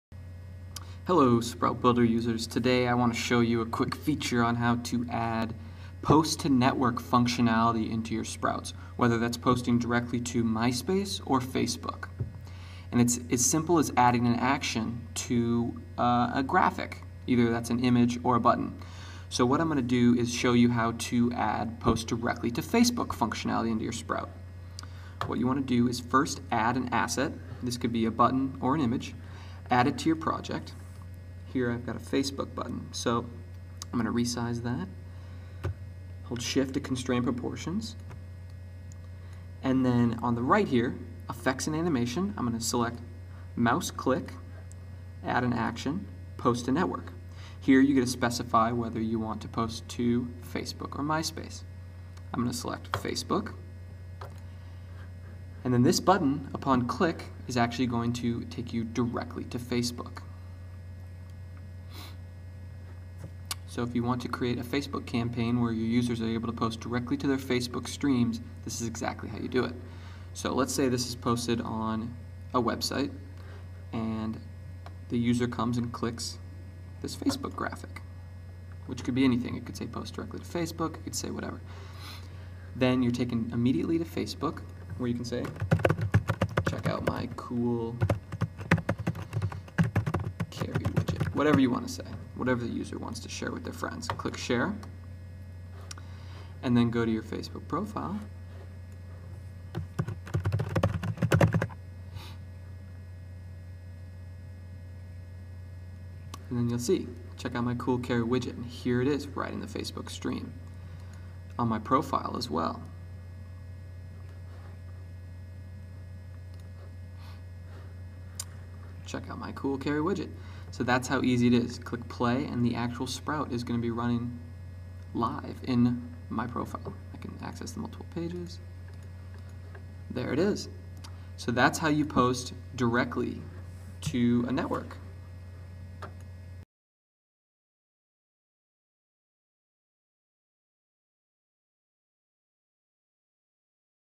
How to add Post-to-Network (MySpace or Facebook) to your sprouts
With Sprout Builder you can create stunning interactive widgets such as the ones seen In this video, I will show you how to add Post to Network functionality directly to your sprouts.  This makes it possible for your users to share sprouts on facebook in a way they are very familiar with.